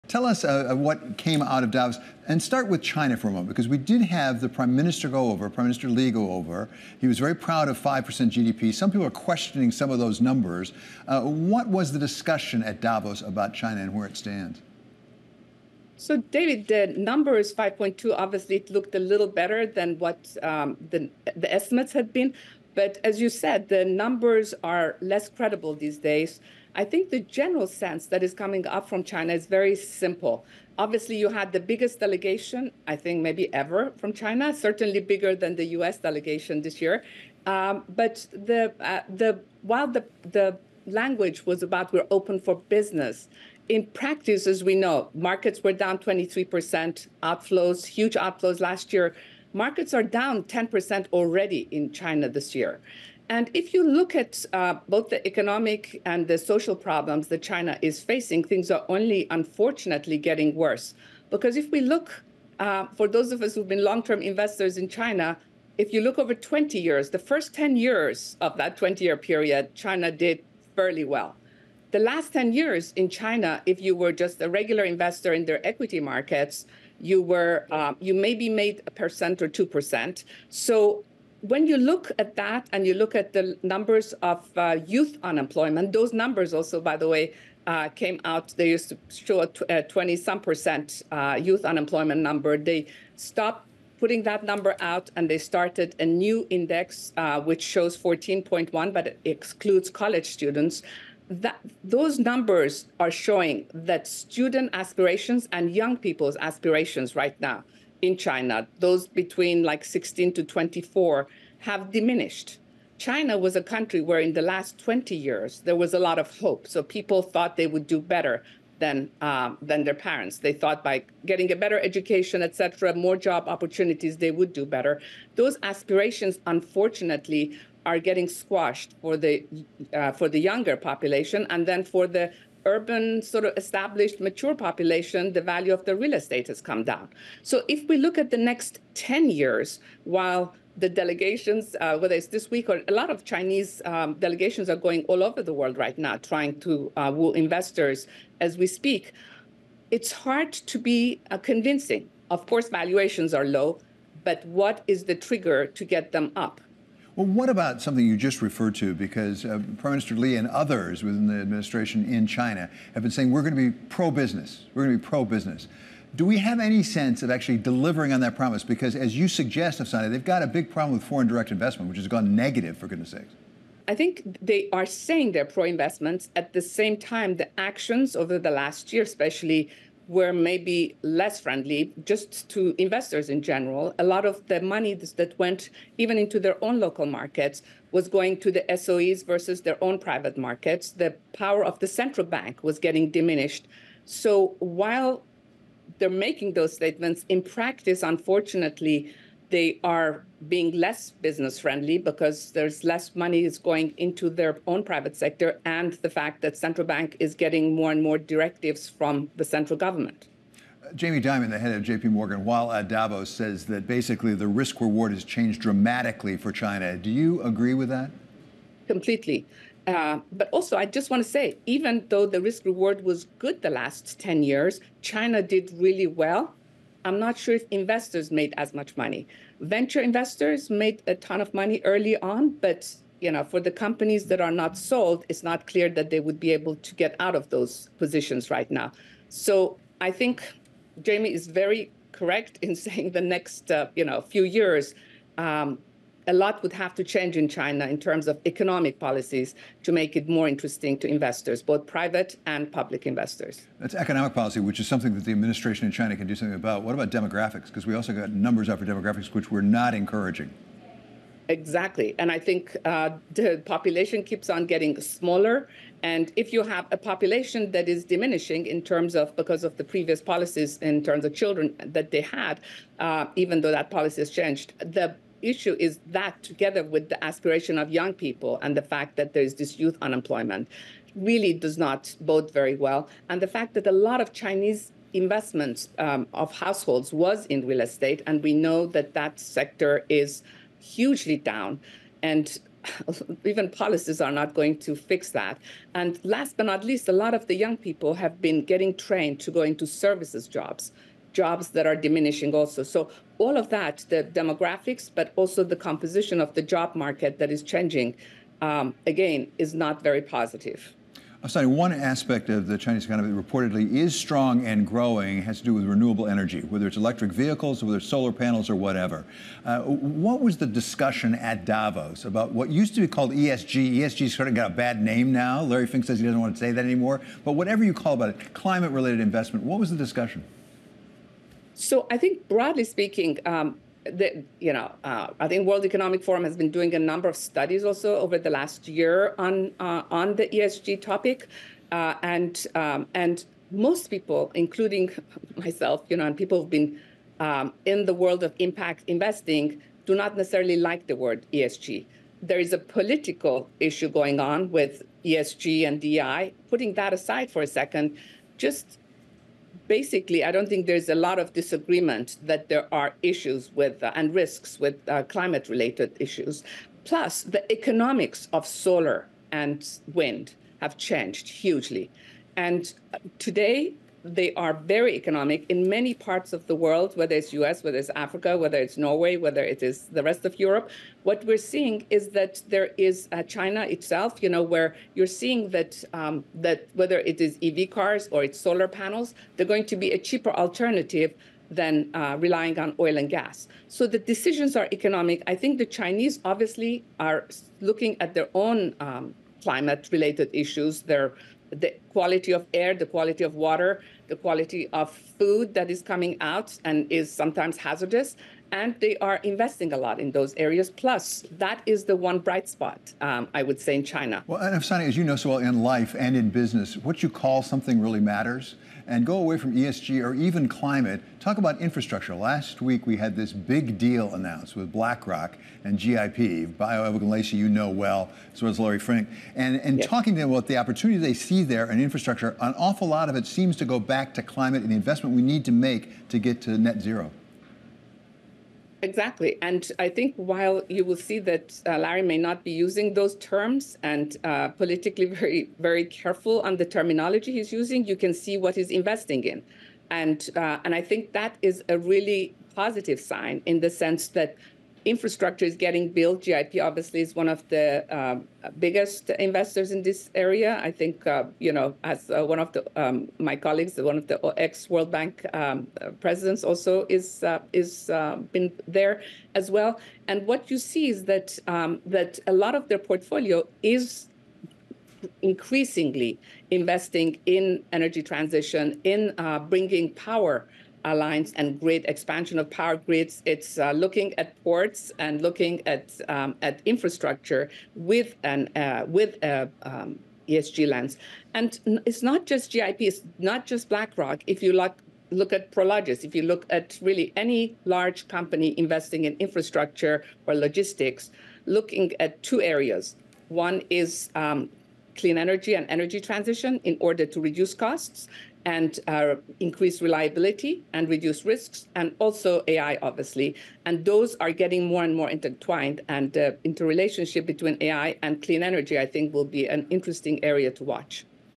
China's Economic Path Forward
Afsaneh Beschloss, RockCreek CEO, thinks China's economic and demographic challenges are worsening. She also explains why the shifting economics of wind and solar energy make climate investing more attractive, even as ESG faces political pressures.